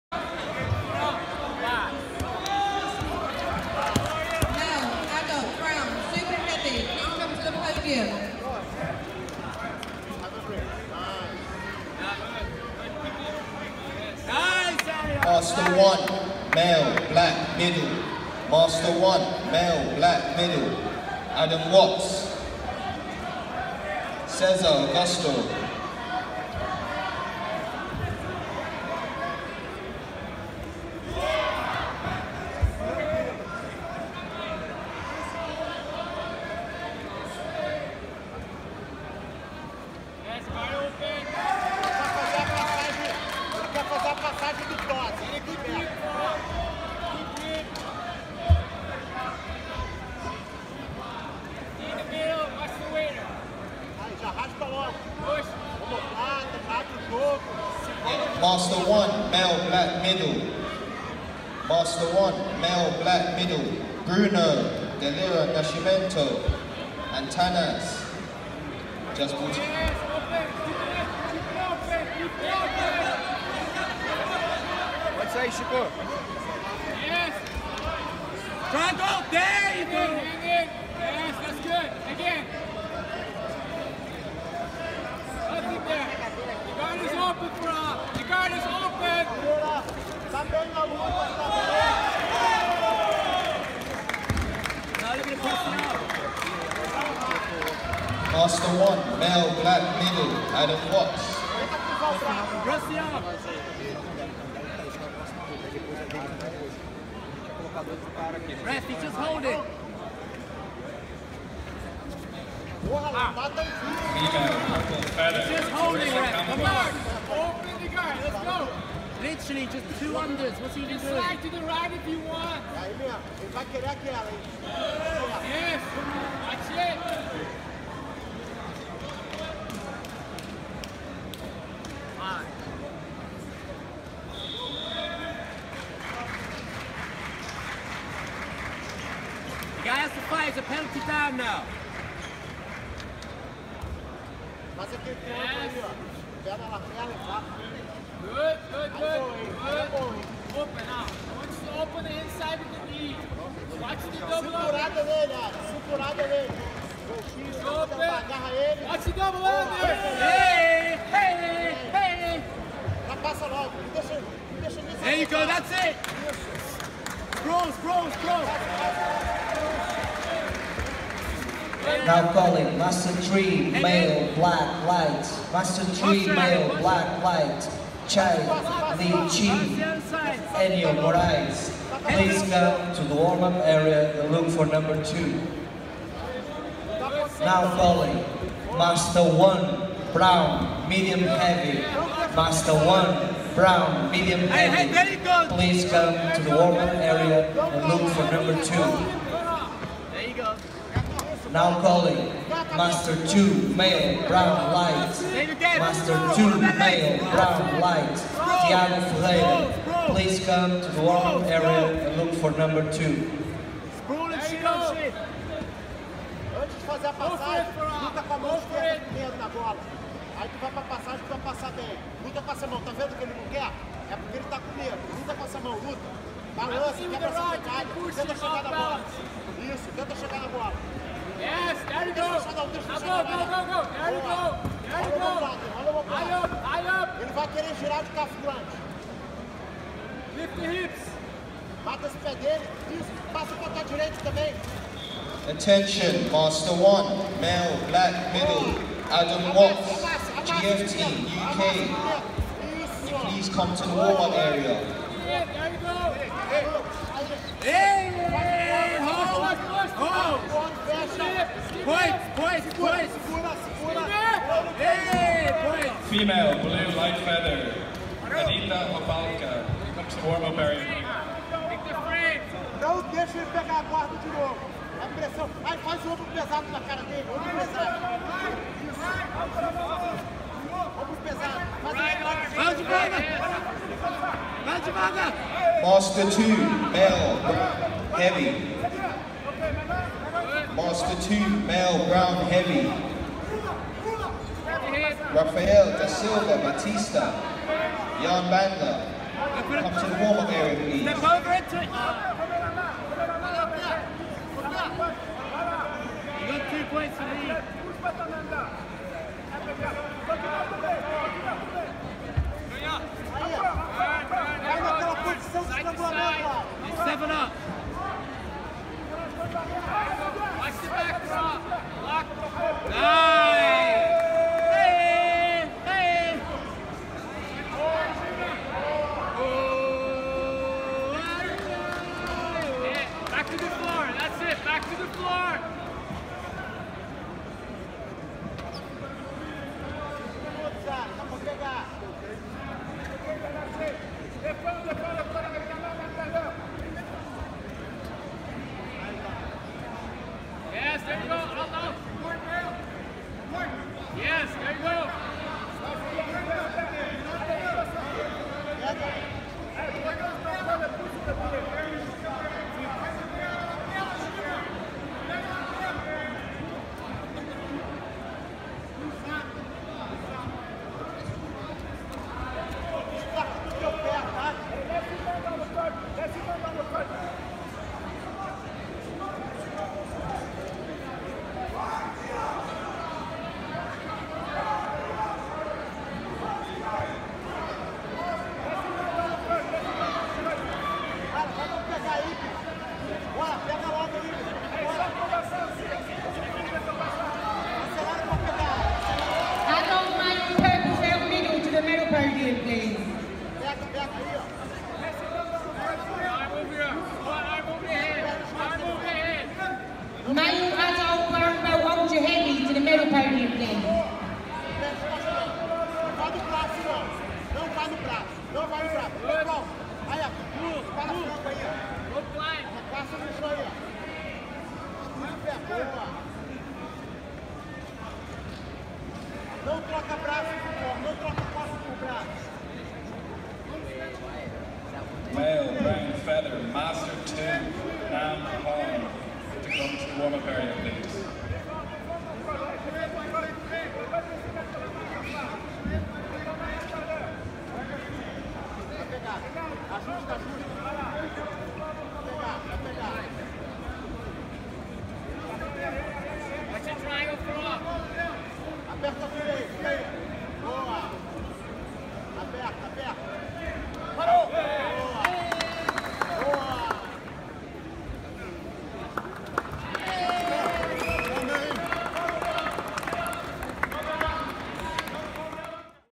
Arya Esfandmaz vs Florent Minguet / British National 2017
Black / Adult / Super-Heavy - Final